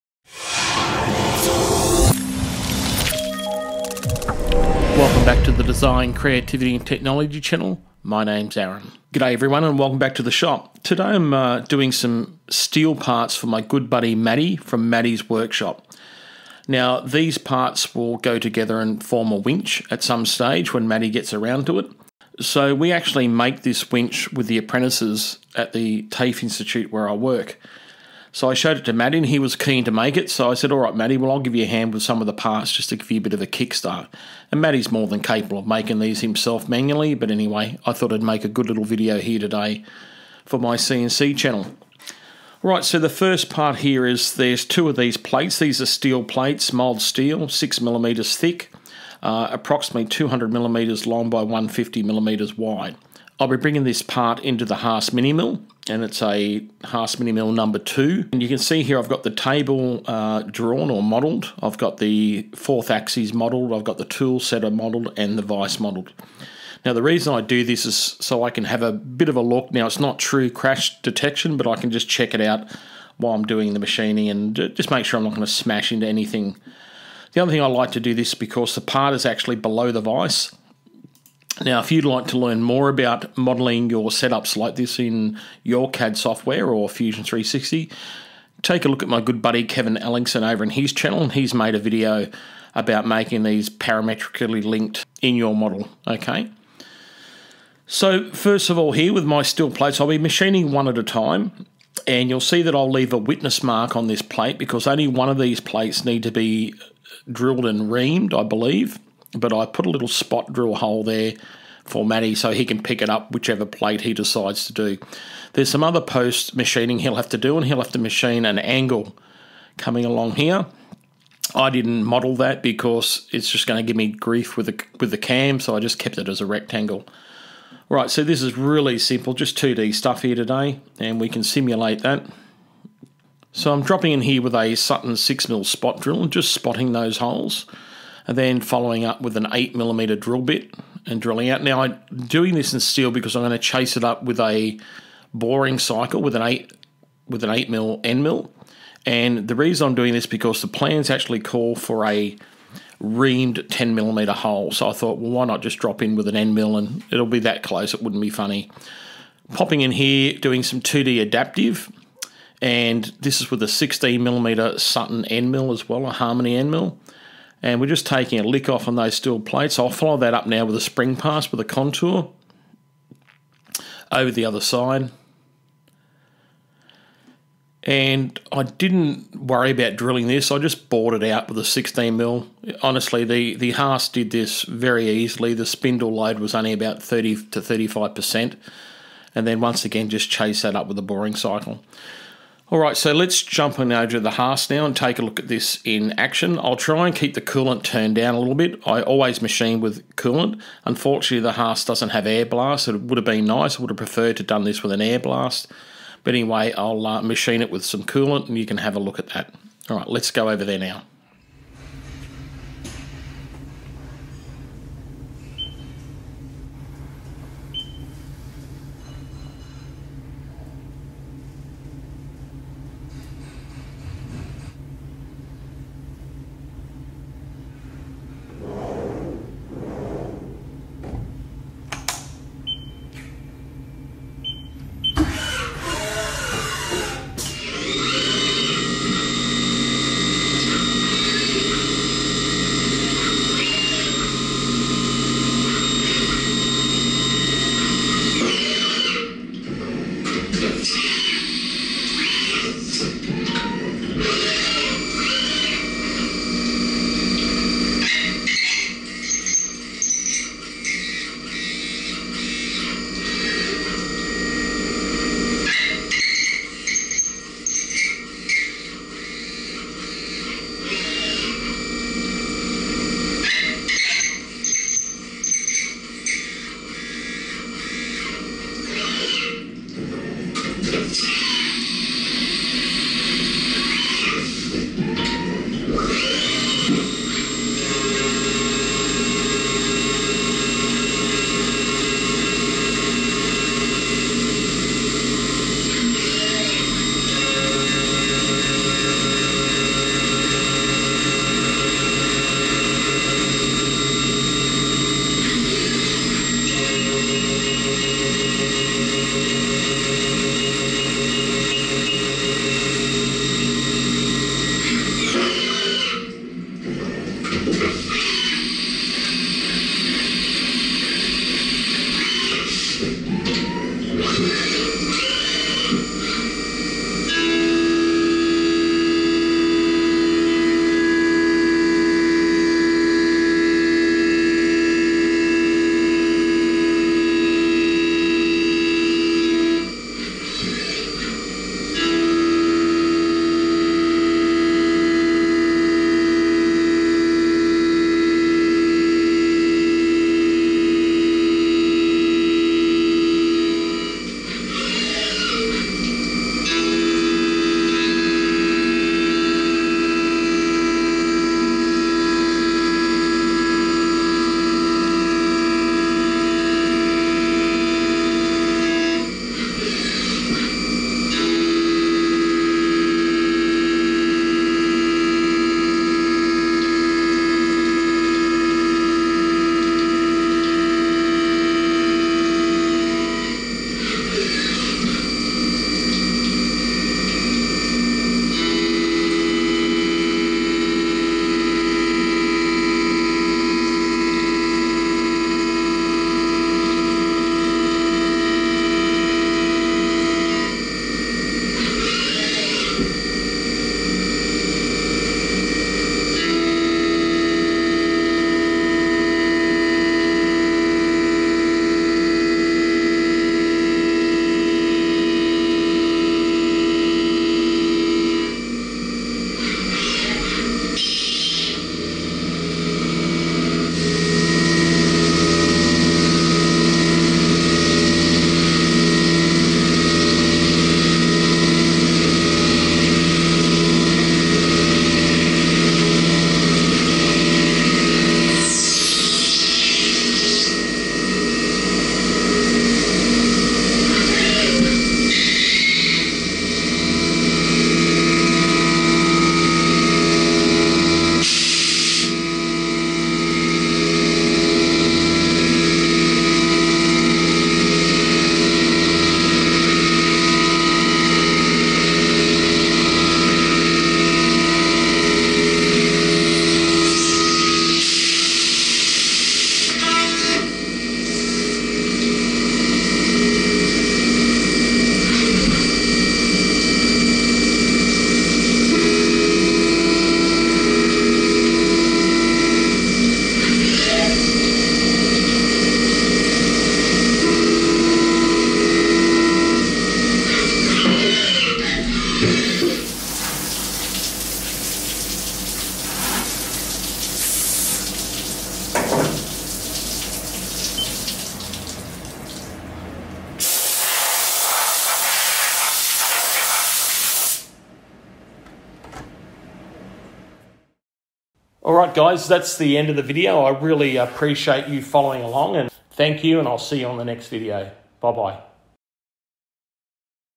CNC parts on the HAAS - Pt.1
This video is part 1 of a collaboration project that I'm doing with my buddy Matty over at Matty's Workshop.  These 6mm thick steel plates are 150 x 200mm long and will form the side plates of the winch that he will be making in an upcoming video.